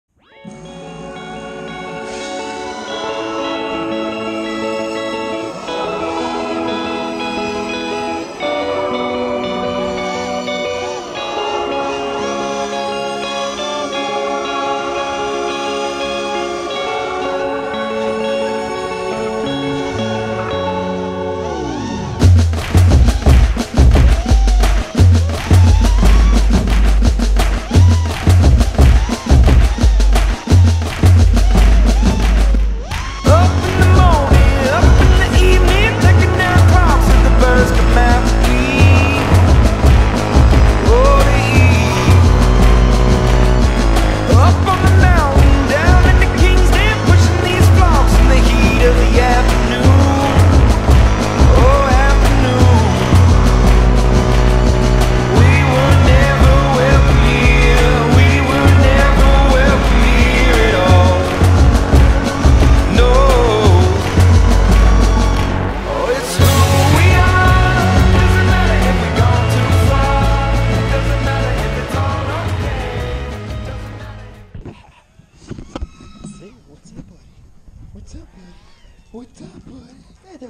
Cottonwood E FPV D4
Some flying today at the park. Great weather in the middle of winter.